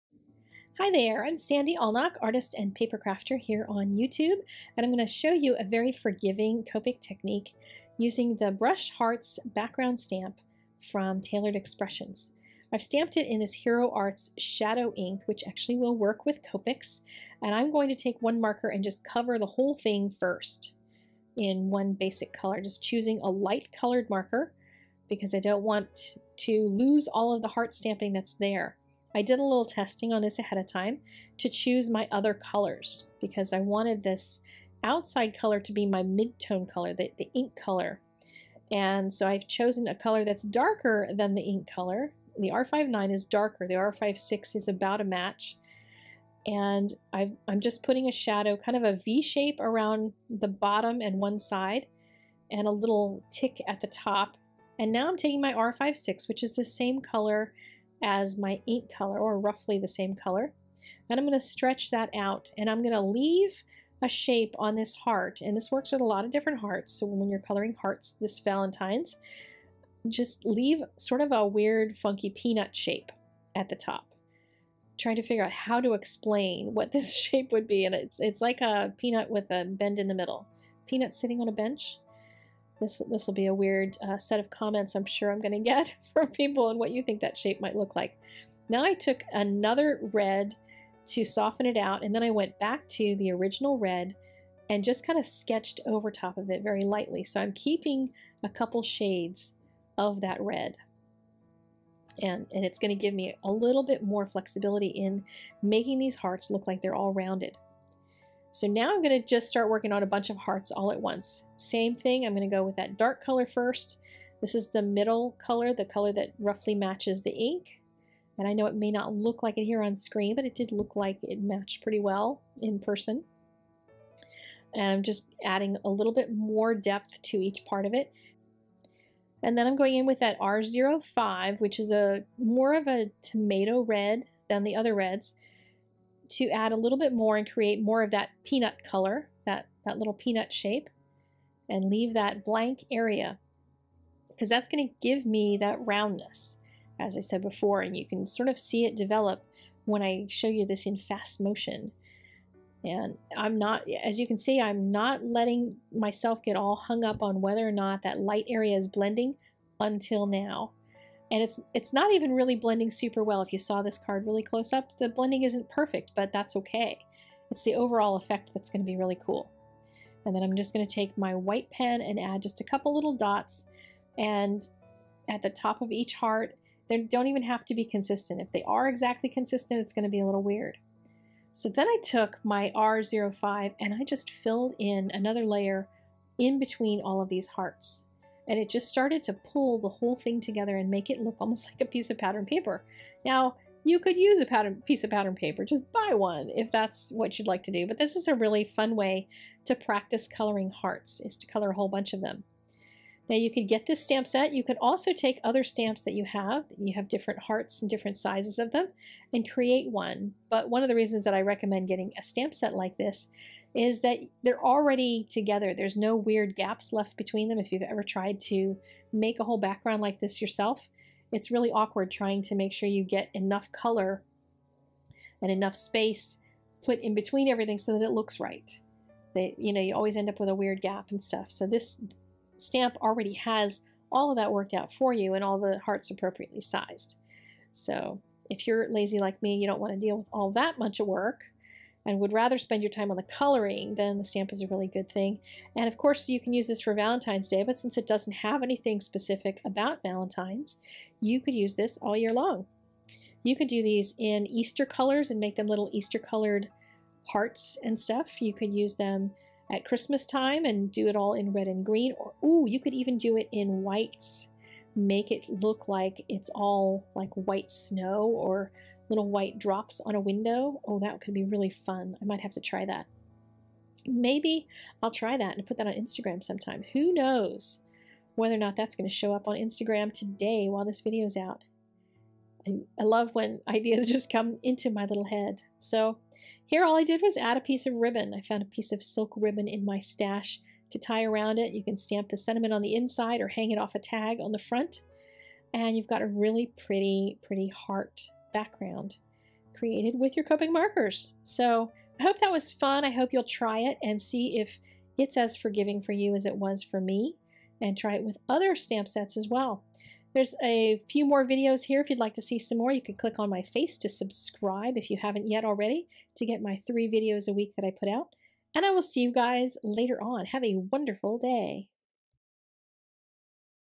Forgiving but EASY No Line Copic Background Technique
I know so many of you say the reds bleed on you - well guess what? This technique relies on that! lol. When in Rome, ya know?